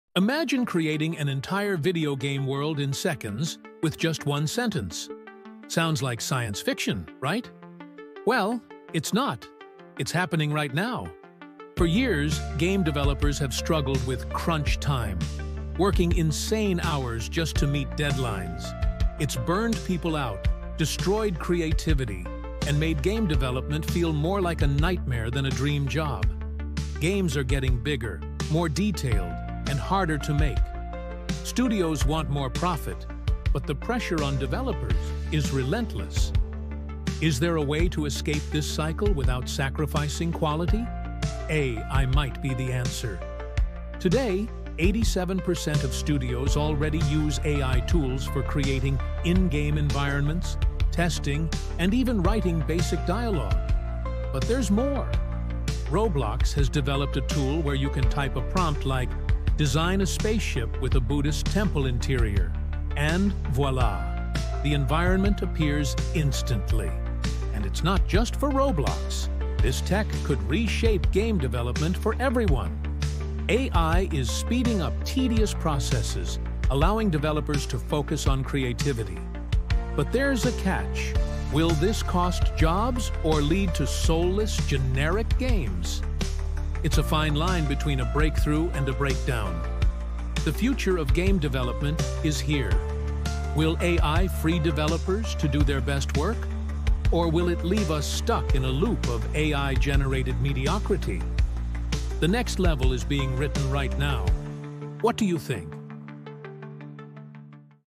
🌟 The Future of Gaming Is Here! 🎮✨
Did you know AI is revolutionizing video game development? From creating entire worlds in seconds to reducing the dreaded crunch time, this tech is changing everything! Imagine typing a single sentence and instantly generating a spaceship or an alien jungle 🌌🚀. But is this the breakthrough developers need—or the end of creativity? 🤔 Watch to find out!